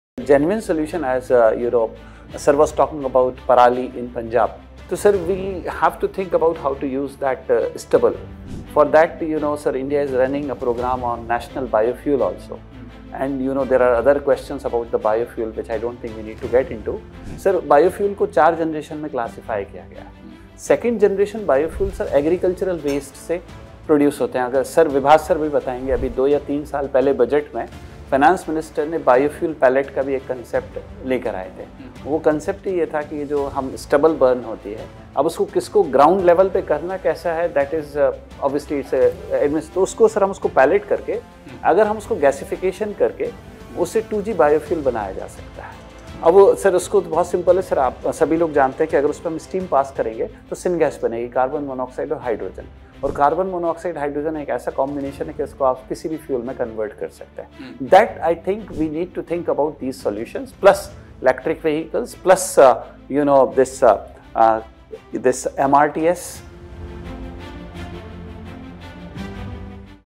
Smart Solutions to Air Pollution | NEXT IAS
In this short talk, Ayaz Sir shares genuine and practical ways to tackle India’s growing air pollution crisis. From turning crop residue (parali) into biofuels to promoting electric vehicles and modern transport systems, he highlights how technology and sustainable planning can together create a cleaner future. 🌱

Let’s think beyond complaints — towards real, workable solutions!